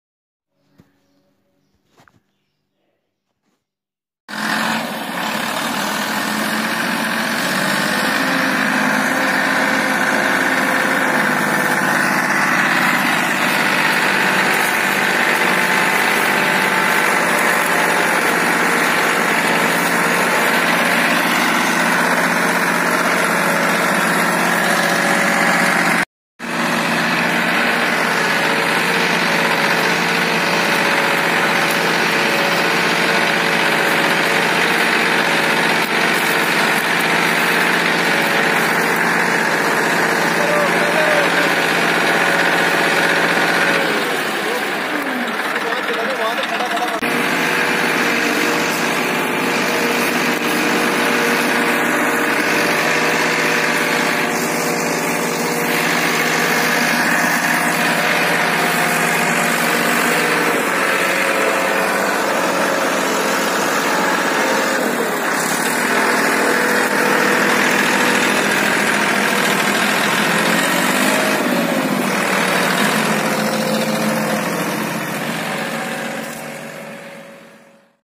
Massey Ferguson 241 || Loads Sugarcane
Massey Ferguson 241 loads quintal's of Sugarcane in India.